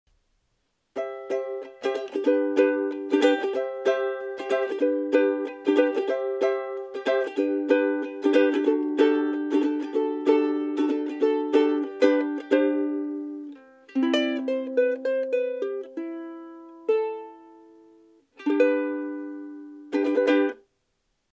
Lanikai ACST-S Soprano Ukulele | The Ukebug
This little Lanikai soprano has been a very pleasant surprise for us here at The Ukebug. With a solid Acacia top and Acacia back and sides it has a visual appeal that continues to draw us in. The Maple binding and single-band Maple inlay rosette pair beautifully with the Acacia wood grain.

We love what Lanikai has done with their satin finish as well as their slightly wider nut and neck profile for improved player comfort. This model, like all the Lanikai ukuleles, comes with chrome strap buttons pre-installed should you choose to use a strap.

Once we moved beyond the initial excitement about the look of this soprano and actually played it, it sure did not disappoint. The volume and depth of tone truly round out the whole playing experience.

With a gig bag included and a limited lifetime warranty at such an accessible price point – we couldn’t possibly be happier. This soprano is sure to please!